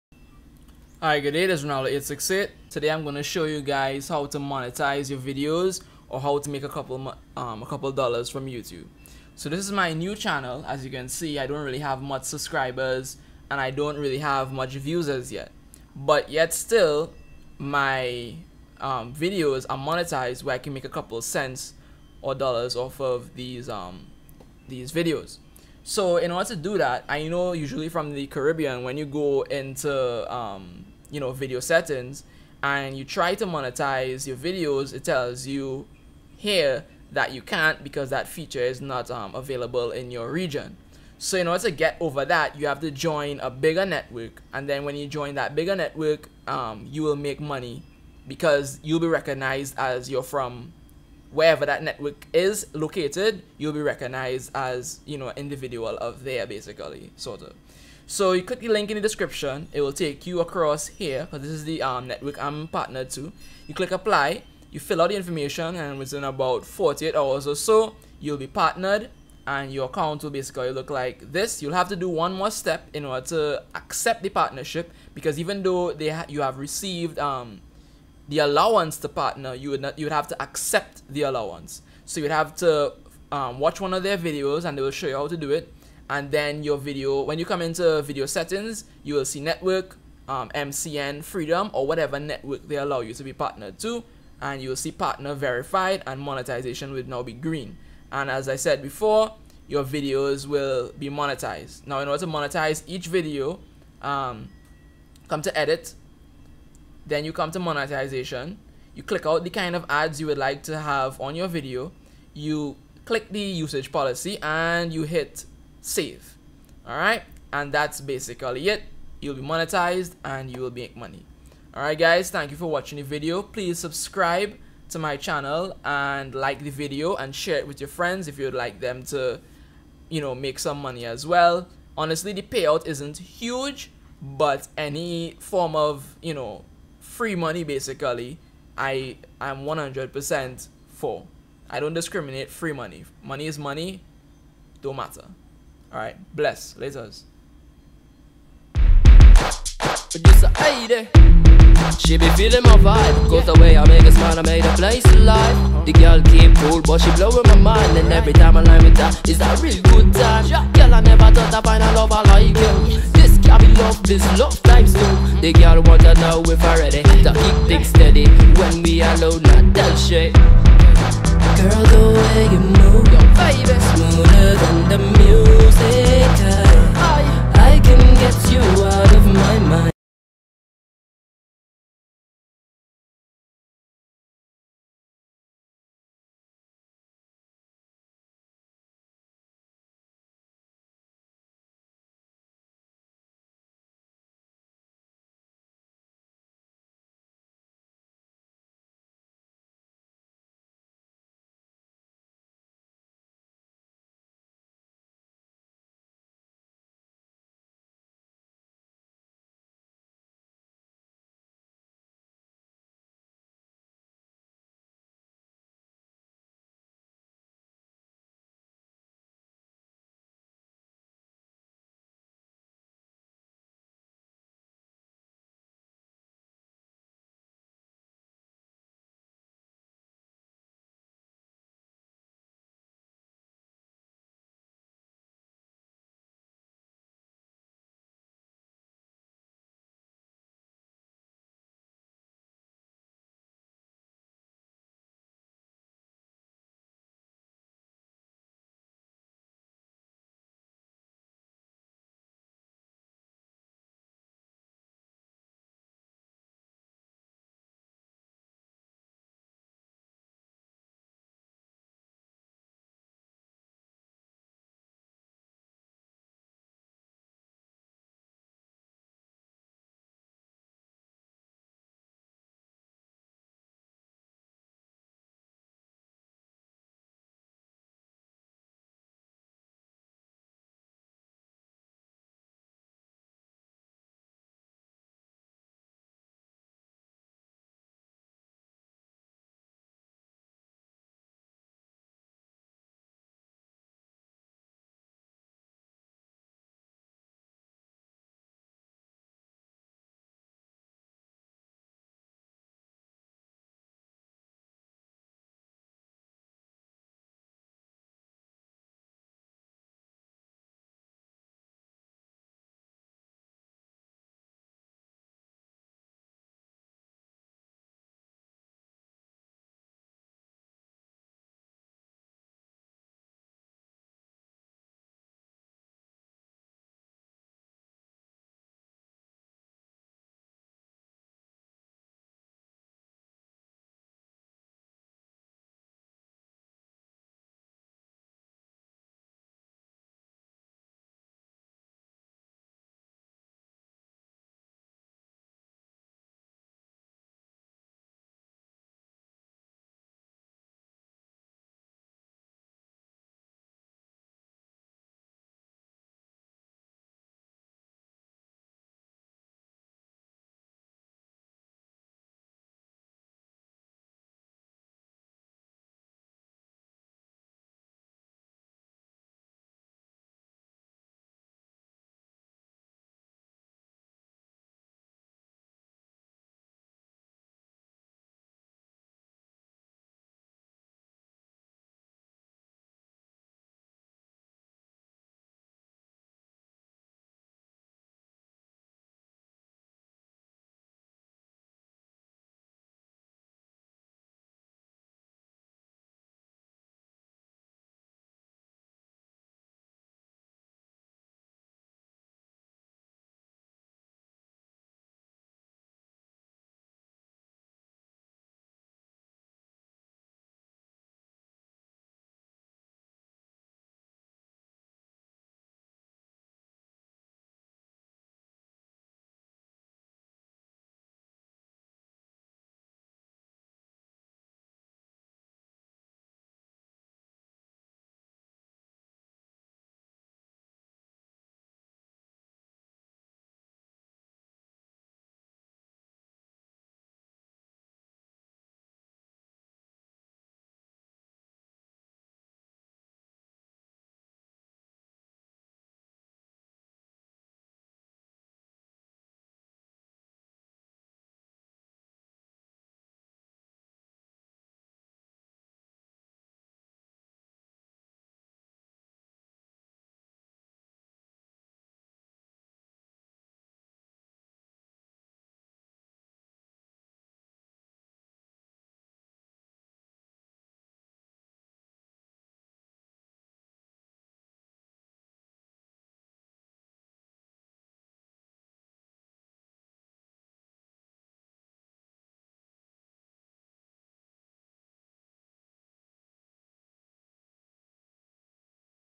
How To Monetize Your YouTube Videos
Learn how to monetize your YouTube videos and make a couple dollars while also building your Subscribers and Views

I forgot to say that you need to place your PayPal email on your new freedom account and if you're from Trinidad and Tobago simply get a VISA travel card or any credit card for that matter and create a PayPal account with it.